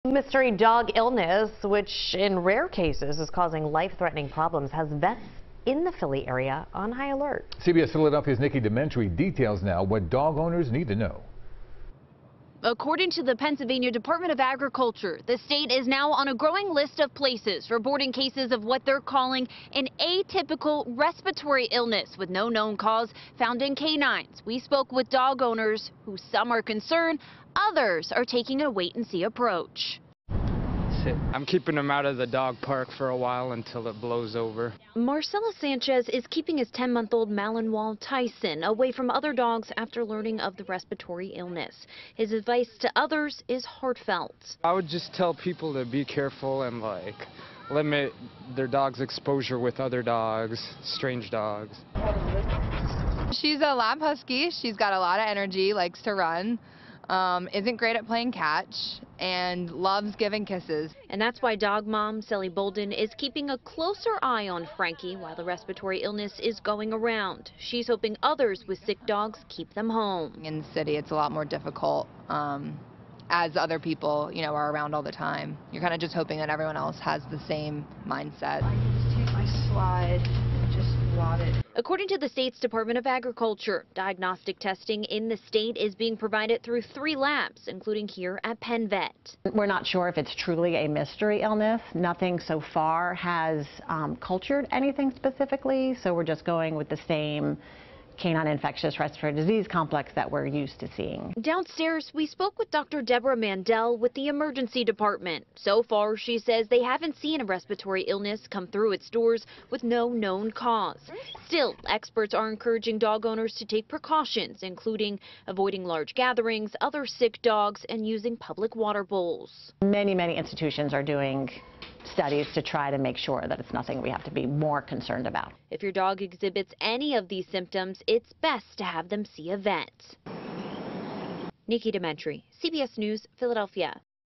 Vets in Philadelphia area on high alert due to mystery dog illness
Cases of the mysterious respiratory illness affecting dogs have been reported in Pennsylvania, the commonwealth's Department of Agriculture said.